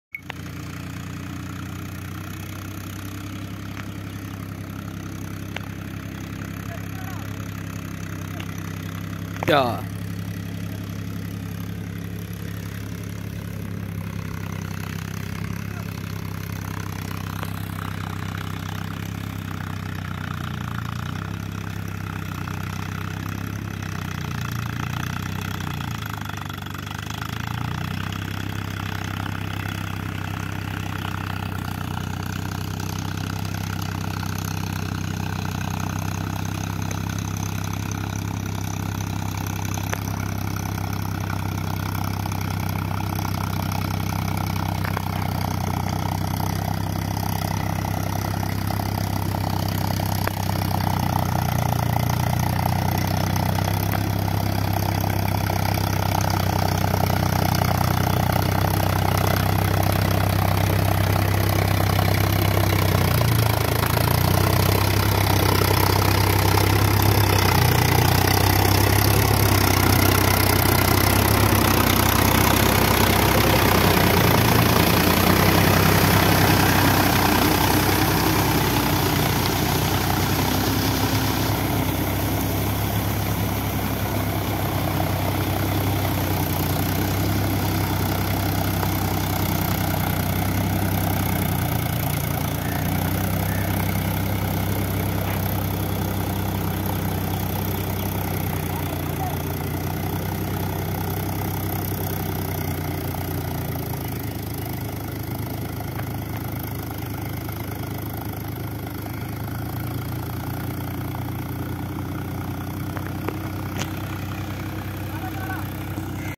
Powertrac Euro 50 Next Series Tractor || 2023 Powertrac Tractor full Review, Power, Performance
Powertrac Euro 50 Tractor || 2023
Powertrac Euro 50 2023
Powertrac Euro 50 New Model 2023
Powertrac Euro 50 New Model tractor 2023

Jagdish Verma G